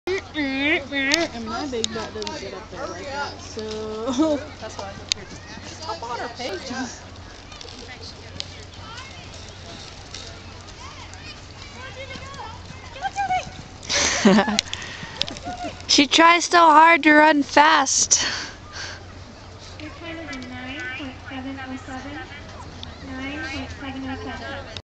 Welsh Pony Flag Racing
Ok I had a loves to run the patterns!! I love taking her!! She likes to think shes as big as the other horses it cute :)